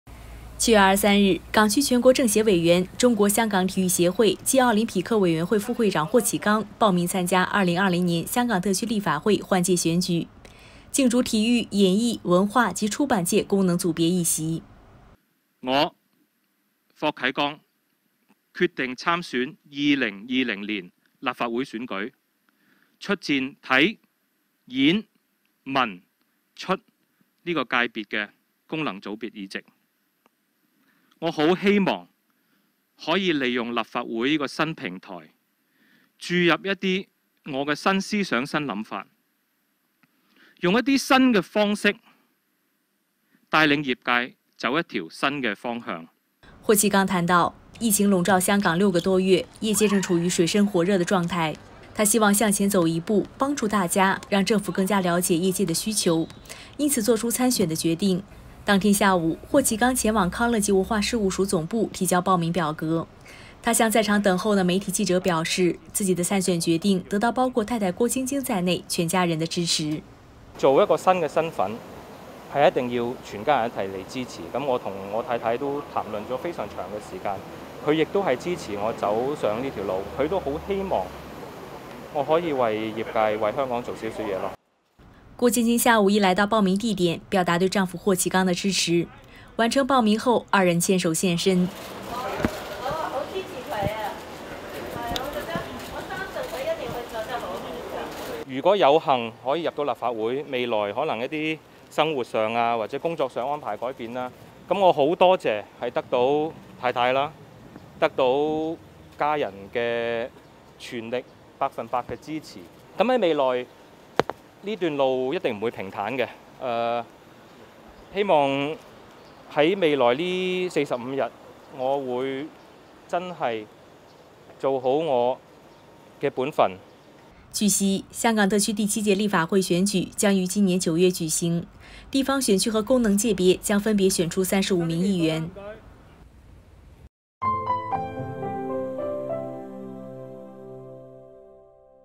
郭晶晶现身支持霍启刚竞逐立法会
7月23日，港区全国政协委员、中国香港体育协会暨奥林匹克委员会副会长霍启刚报名参加2020年香港特区立法会换届选举，竞逐体育、演艺、文化及出版界功能组别议席。
       霍启刚谈到，疫情已经笼罩香港6个多月，业界正处于“水深火热”的状态，他希望向前走一步，帮助大家，让政府更加了解业界的需求，因此作出参选的决定。
        当天下午，霍启刚前往康乐及文化事务署总部提交报名表格。他向在场等候的媒体记者表示，自己的参选决定得到包括太太郭晶晶在内全家人的支持。